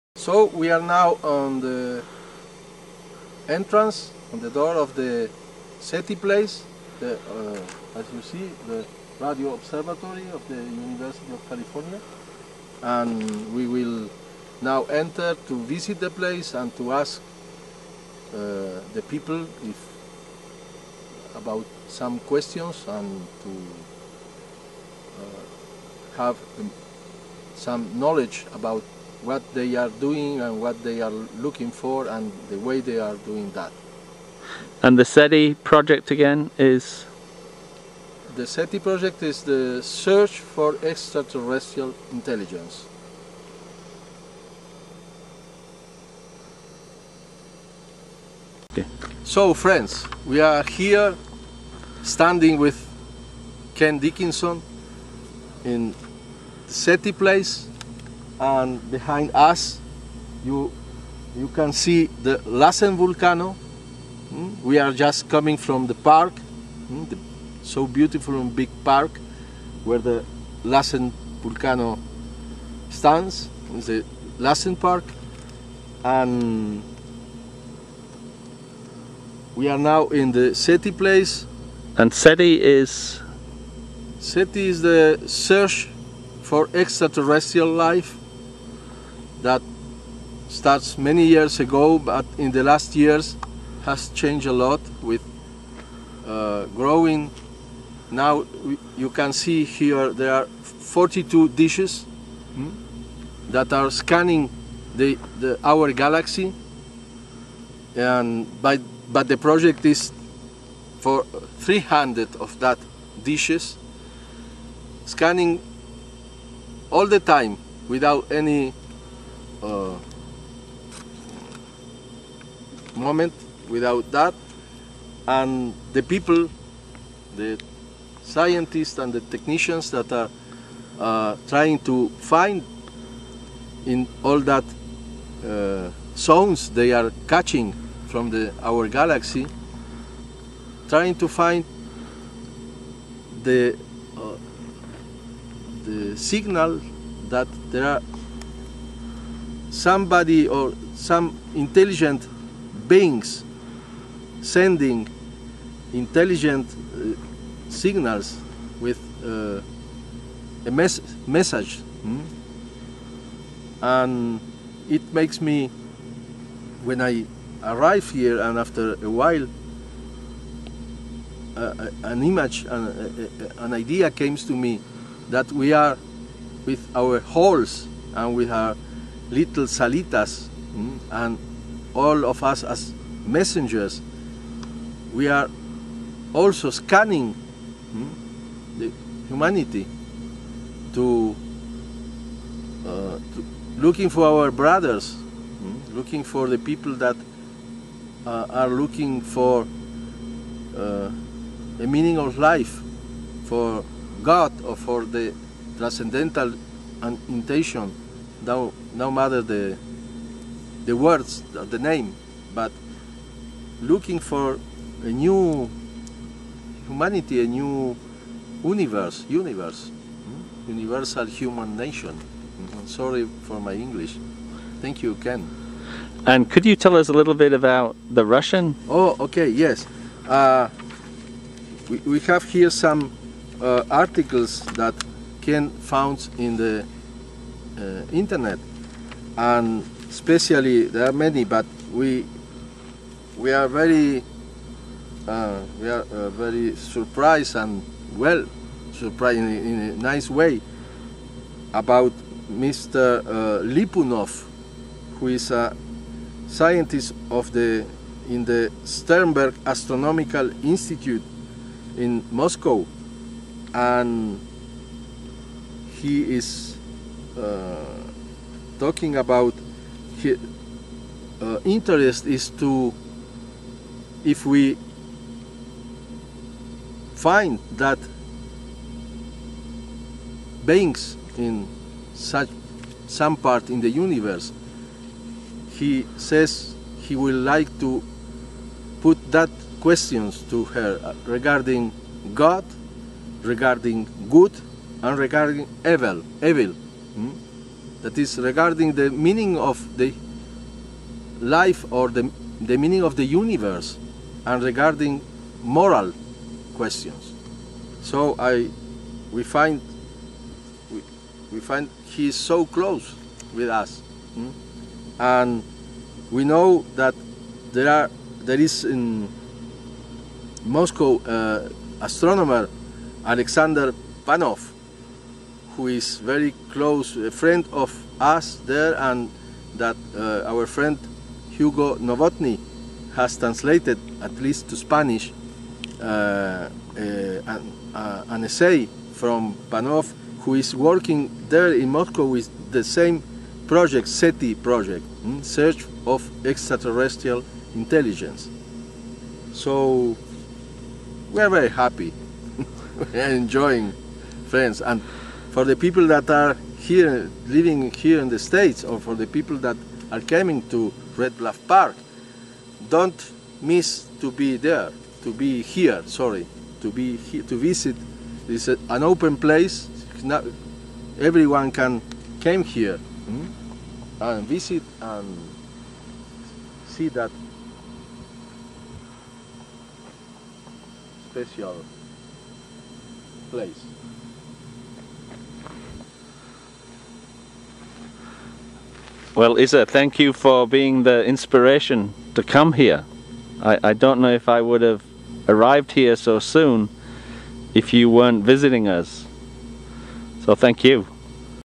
SETI - Search For Extra Terrestrial Intelligence
A visit with Isaias Nobel to the SETI (Search For Extra Terrestrial Intelligence) site at the Hat Creek Radio Observatory near Red Bluff Park, California USA